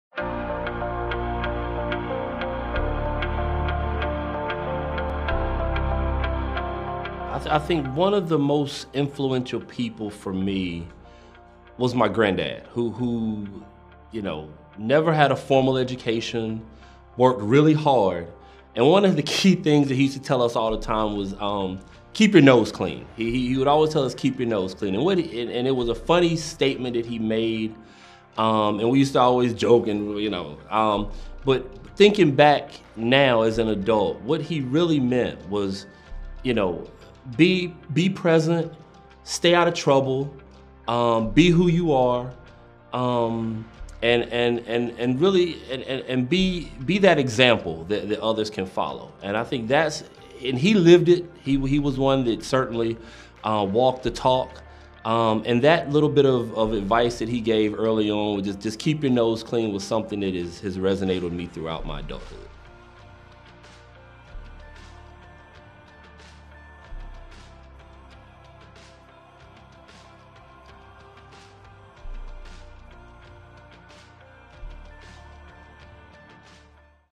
NEW Wisdom - Stephen Leach - Keep Your Nose Clean
As the Manager of Workforce Diversity & Inclusion at Nestle USA, Stephen Leach has lived by the phrase "Keep Your Nose Clean". From a silly phrase he once joked about, to a life lesson he keeps close to His heart, Stephen identifies the importance of staying true to who you are and leading by example.